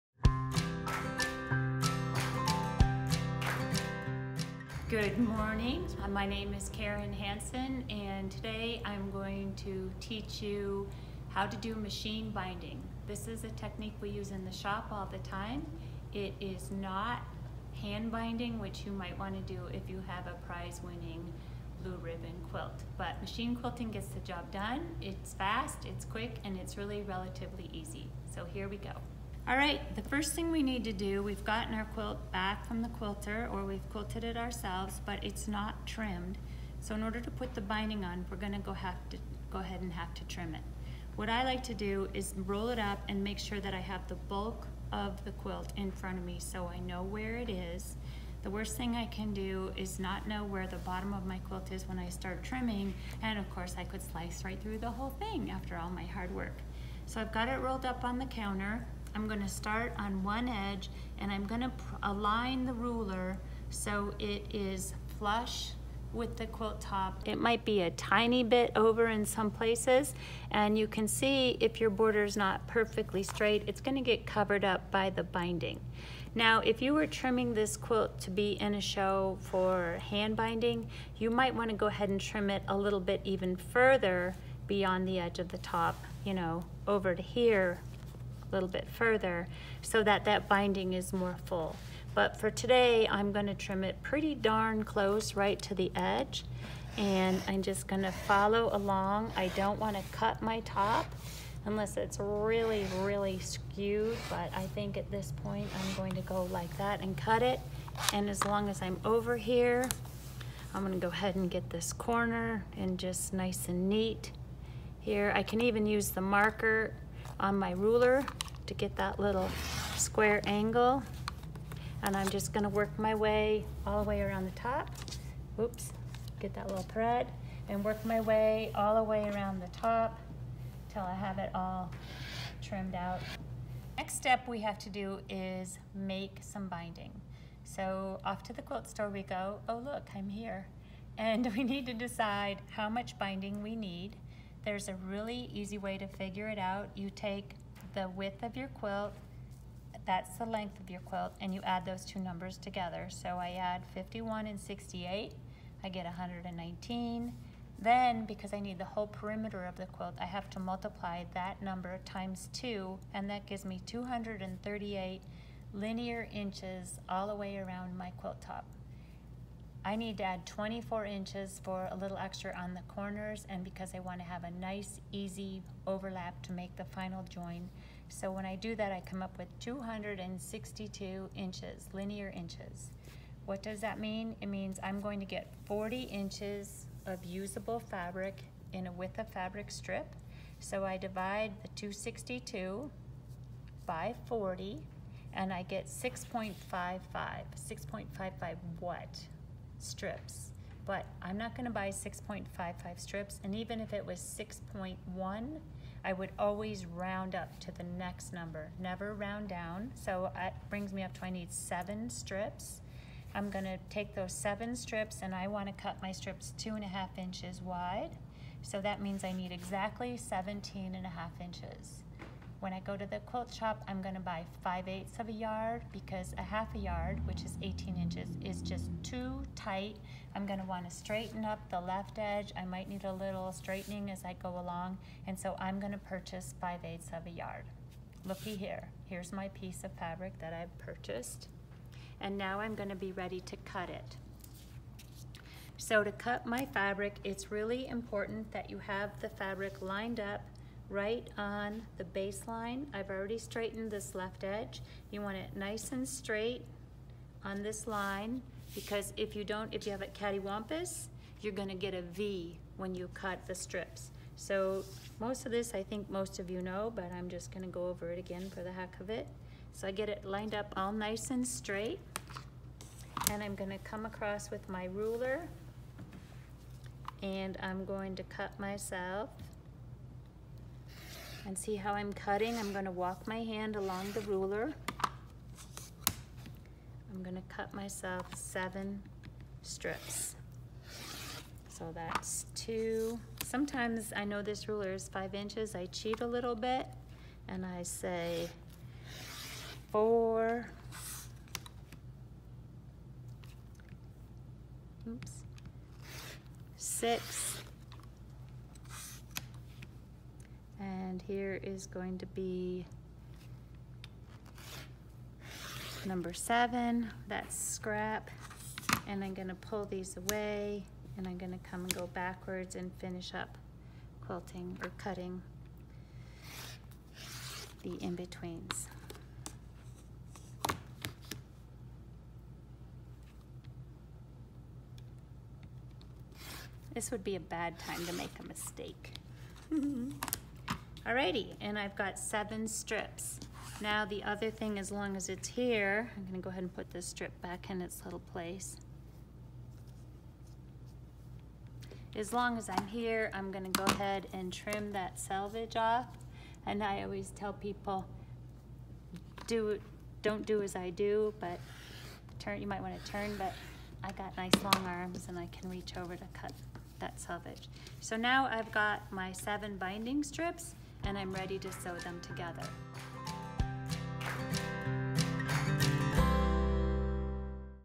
Mrs. Hanson's Perfect Machine Binding Part I
This video is part one of a three-part series. This step by step video shows you how to trim your quilt for binding, how to figure yardage for binding, and how to rotary cut your binding strips. Skip ahead to part three if you're already a binding master.